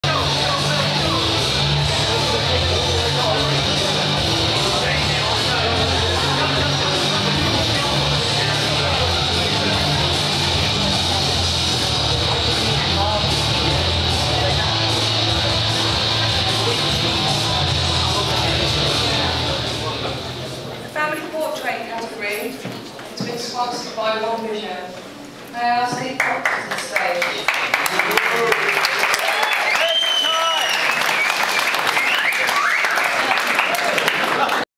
Master Photography Awards with John Swannell 2013
Master Photography Awards 2013/14 at St John's Hotel, Solihull.